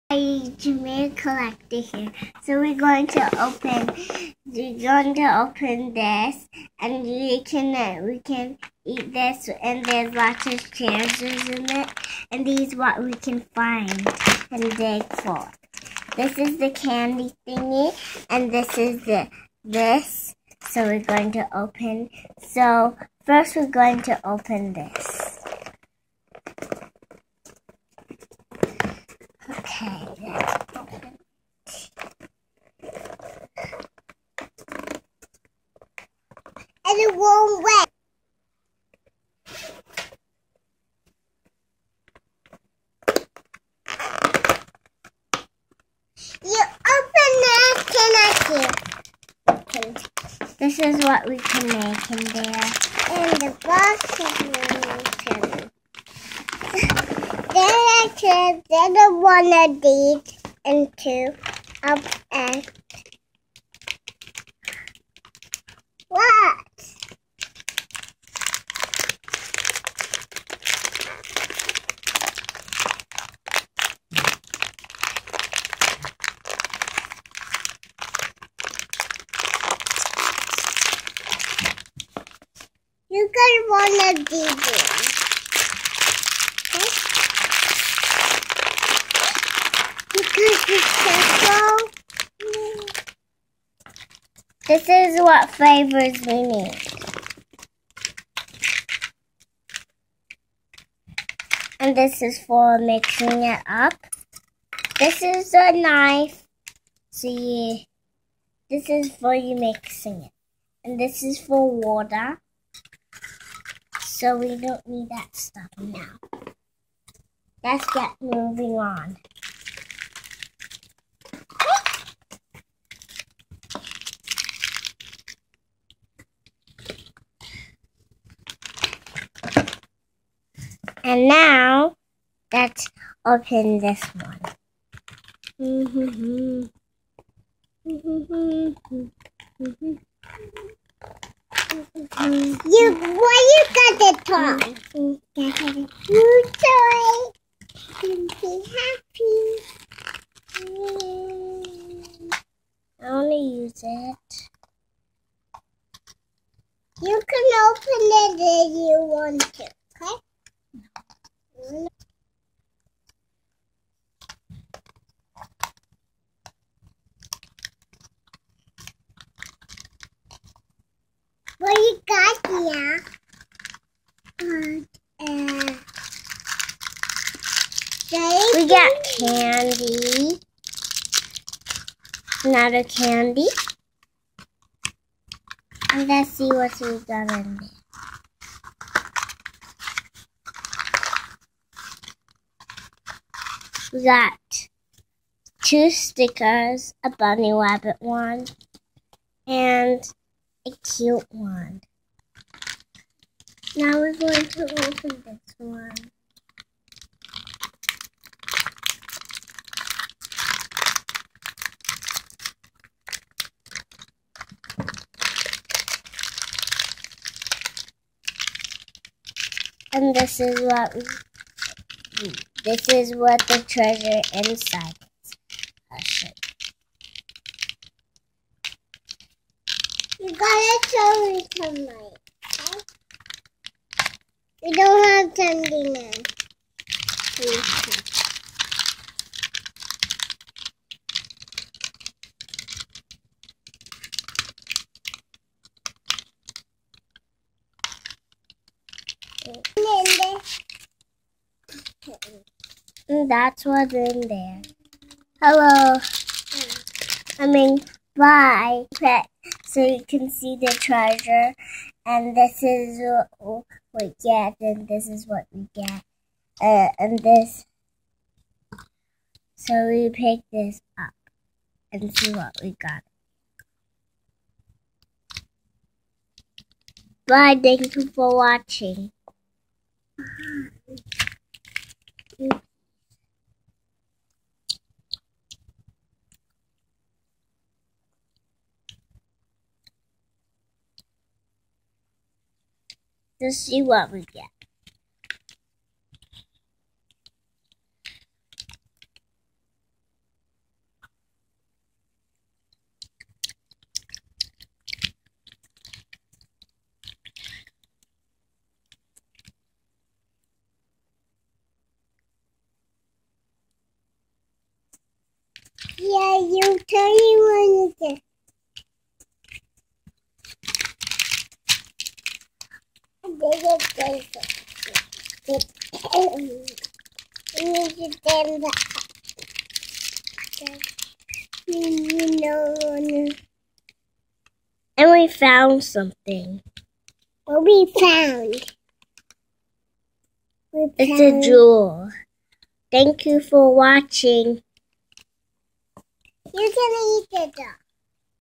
Japanese candy DIY treats
'Elsa' opening Japanese candy treats from Mitsuya supermarket, does own 'elevator' background music, with guest commentary by Mumu. Inspired by 'Disney Collector Br'.